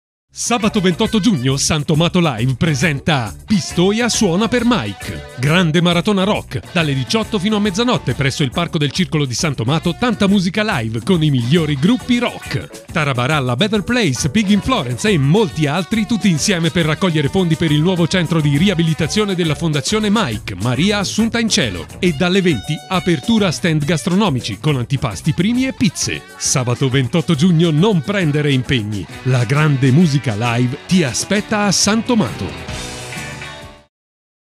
PROMO SANTOMATO PRO MAIC
Spot della maratona musicale benefica ce si svolgerà a santomato il 28 Giugno 2014